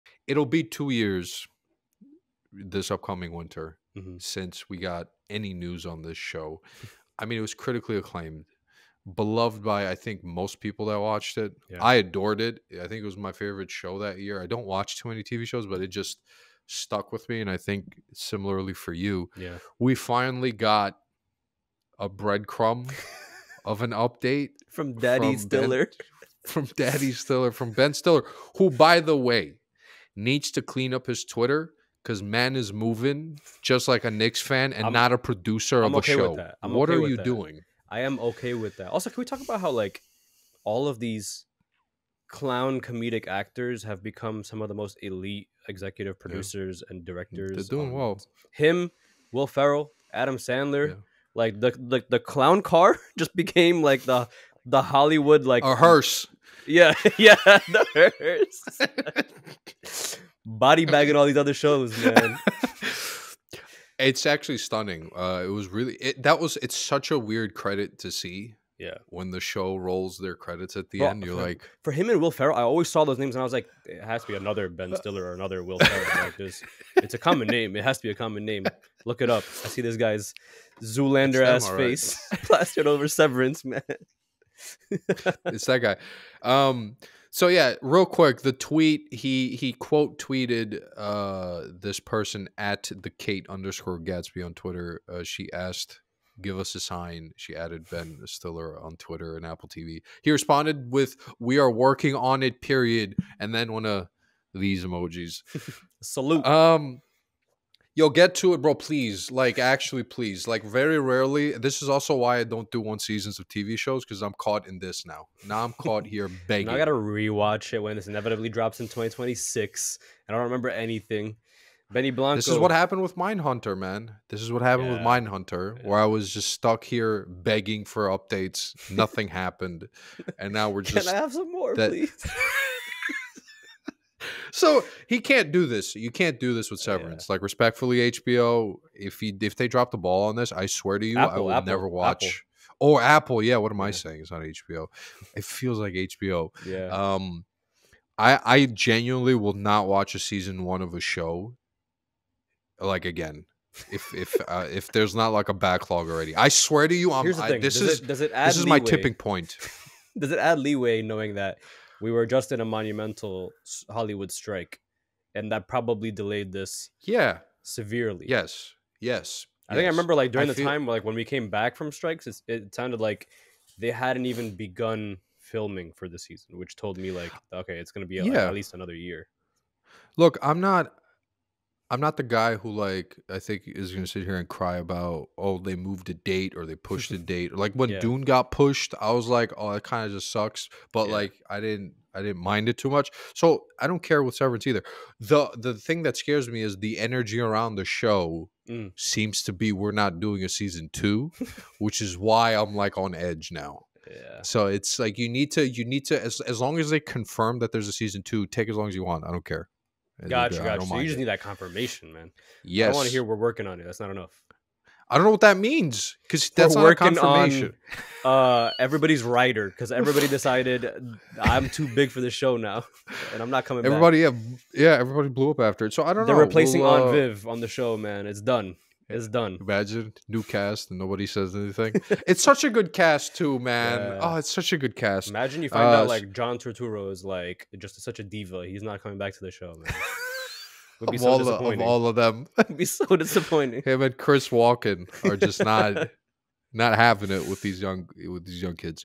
Ben Stiller Updates Everyone on Season 2 of Severance?
The duo discuss a *potential* update from Ben Stiller on the forthcoming season 2 of the beloved Apple+ show Severance.